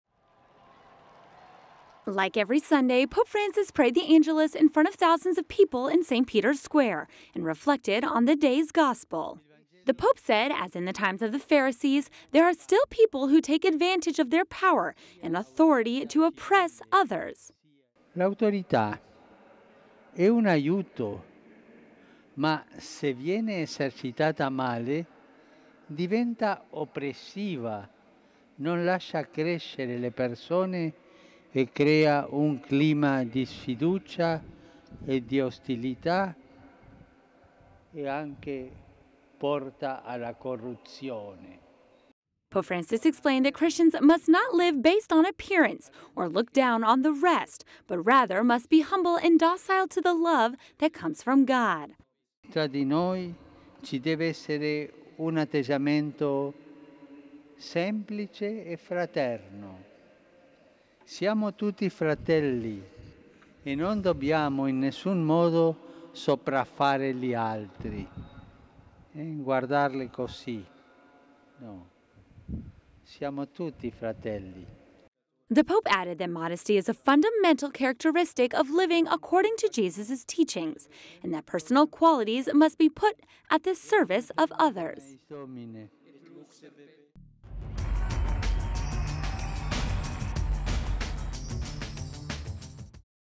Pope Francis in Angelus: “Abused authority leads to corruption”
The pope invited Christians to be humble, following Jesus' example.